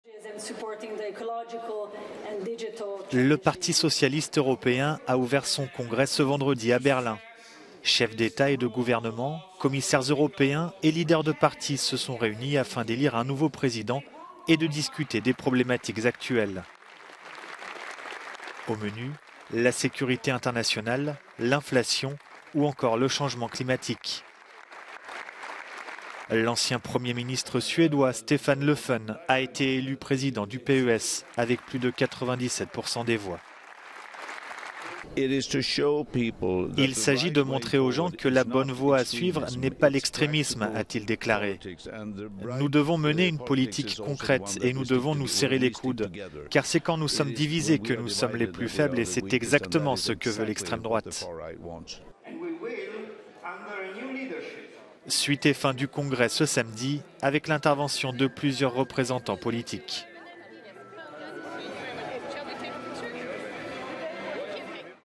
Le Suédois Stefan Löfven élu président du Parti socialiste européen
L'ancien Premier ministre suédois a été élu président du Parti socialiste européen le d'un congrès organisé à Berlin, ce vendredi.